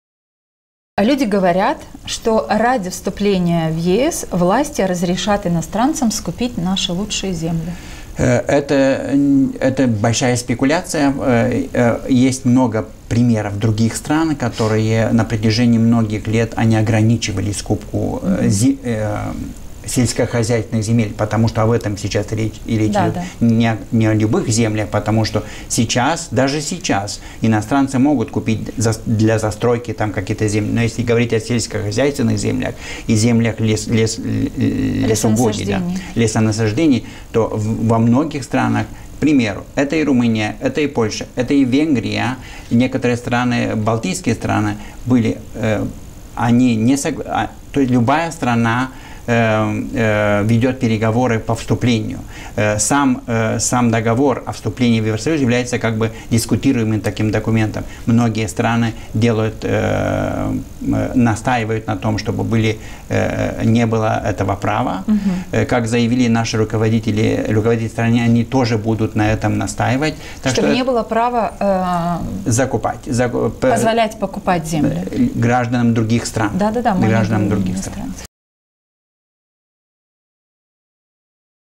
Членство Молдовы в Евросоюзе позволит иностранцам скупать земли?
➡️Своё мнение по этому вопросу в программе «Единомышленники» высказал - национальный эксперт в области развития СМИ и журналистской этики, Петру Маковей.